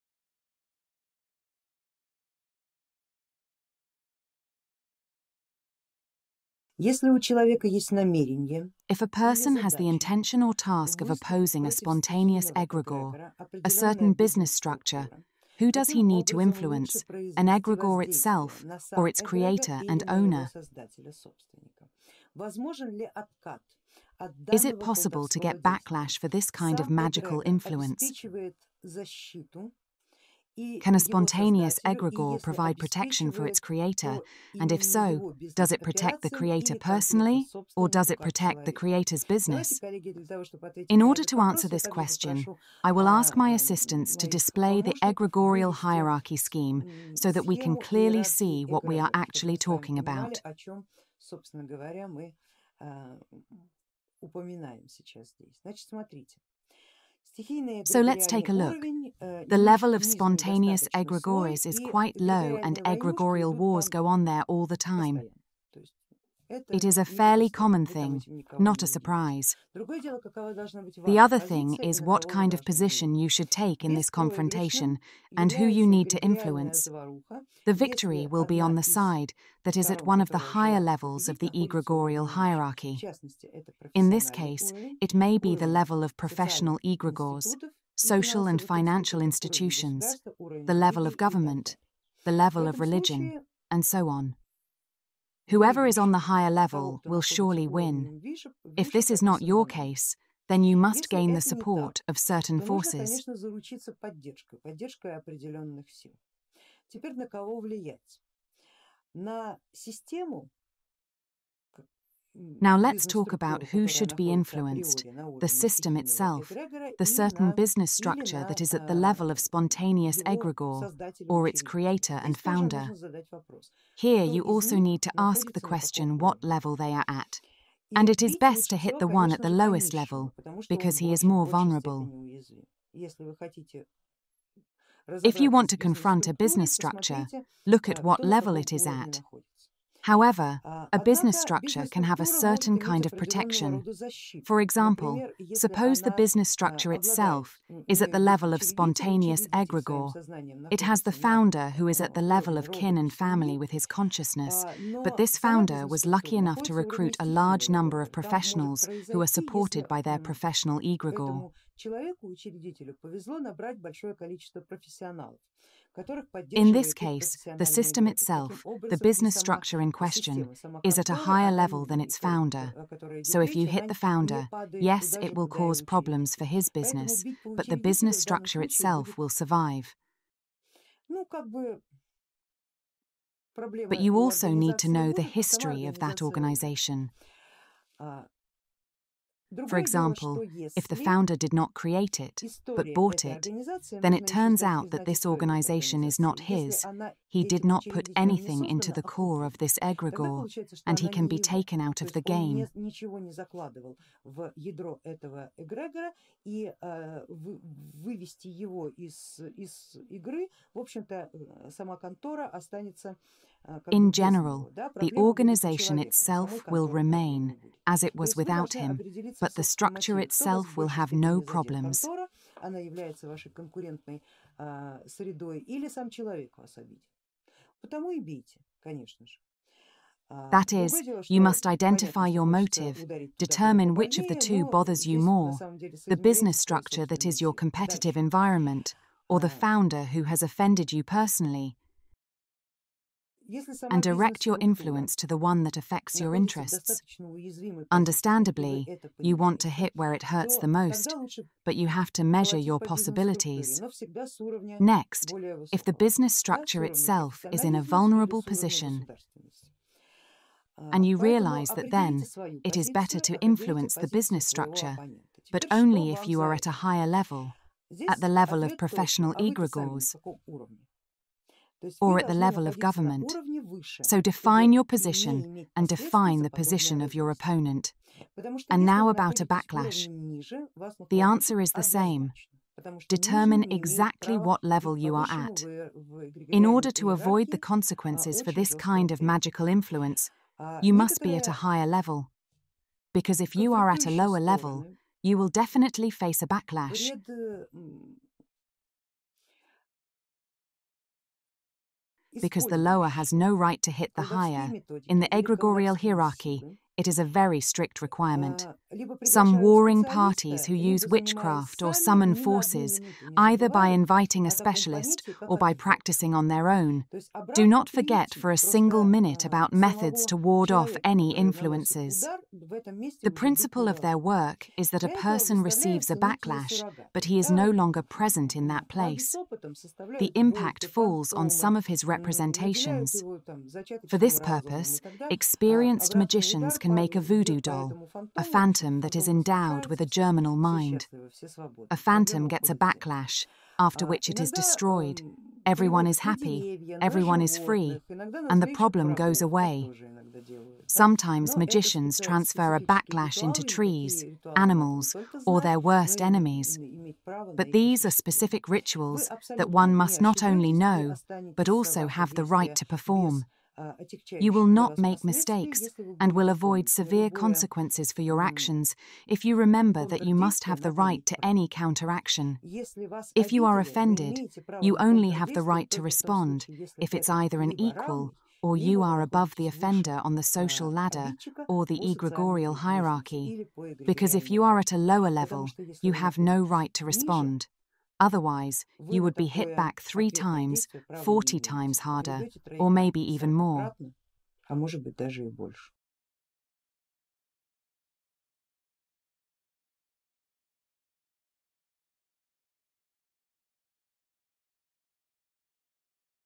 War Between Sorcerer And Egregore
War Between Sorcerer And Egregore. If a person has the intention or task of opposing a spontaneous egregore, a certain business structure, who does he need to influence, an egregore itself or its creator and owner? Is it possible to get backlash for this kind of magical influence? Can a spontaneous egregore provide protection for its creator, and if so, does it protect the creator personally, or does it protect the creator's business?
- The level of spontaneous egregores is quite low and egregorial wars go on there all the time. The other thing is what kind of position you should take in this confrontation and who you need to influence.
The victory will be on the side that is at one of the higher levels of the egregorial hierarchy. In this case, it may be the level of professional egregores, social and financial institutions, the level of government, the level of religion, and so on. Whoever is on the higher level will surely win. If this is not your case, then you must gain the support of certain forces.